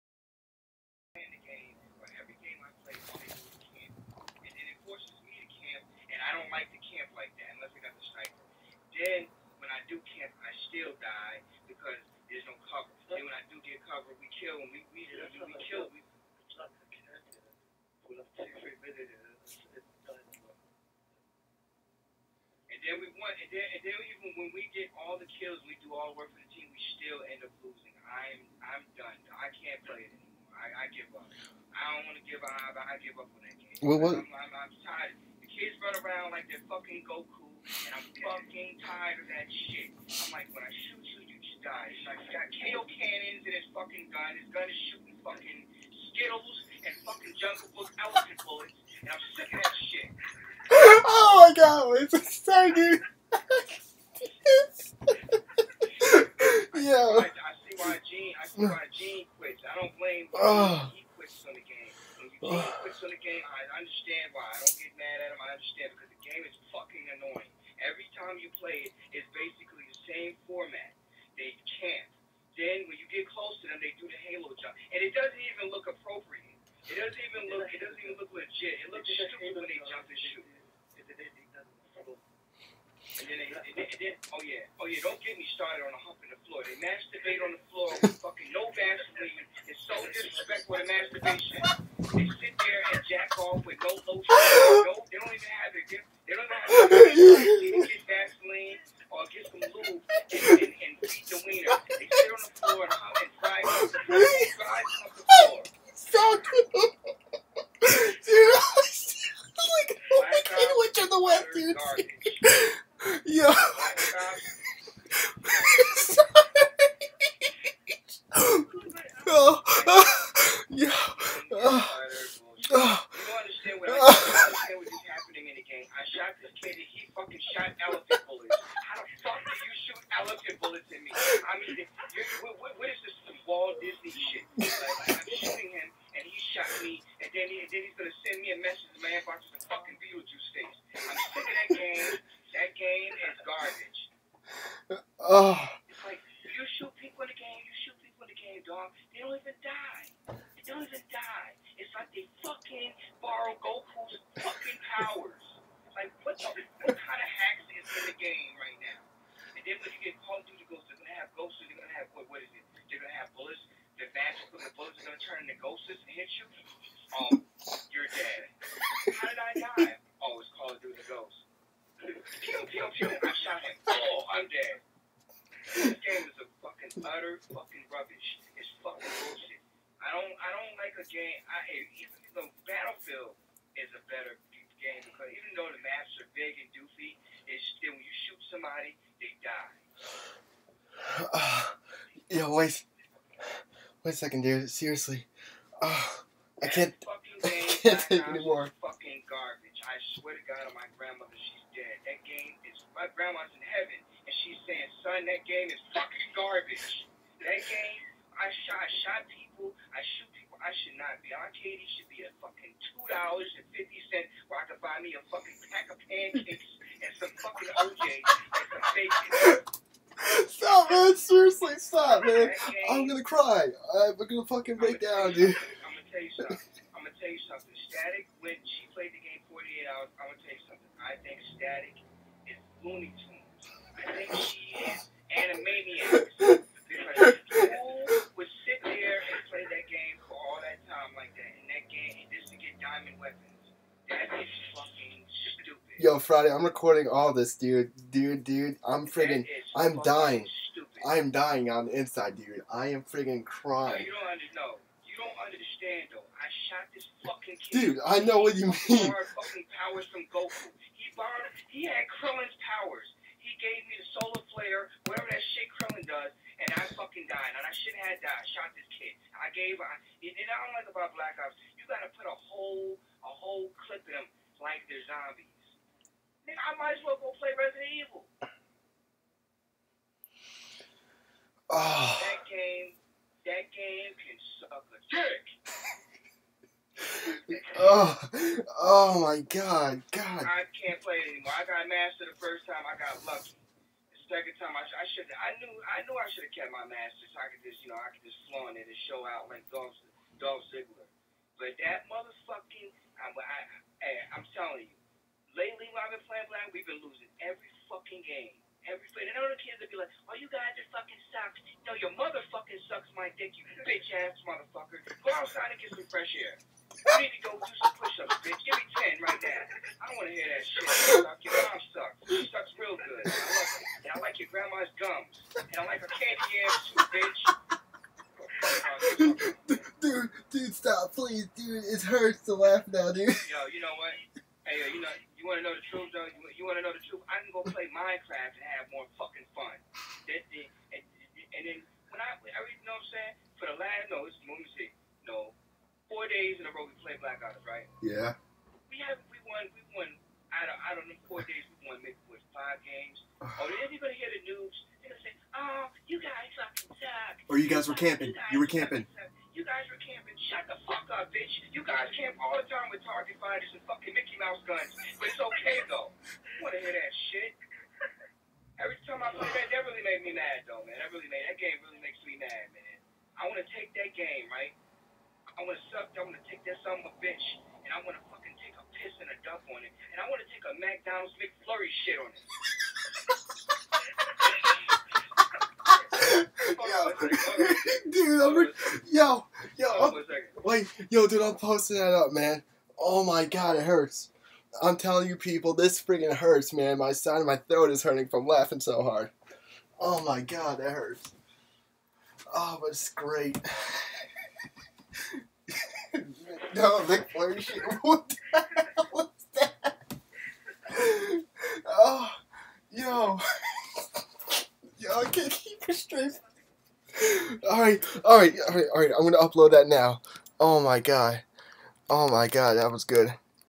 2Nasty Crew Tells The Truth: Camp Of Booty Crack Ops 2
People Im sorry but I was laughing so hard that I fell on the ground and sorry if Im shirtless I was in Pajama's. My CakeBook Page. Profile pic: Las Lindas. Camp Of Booty Bullshit 24/7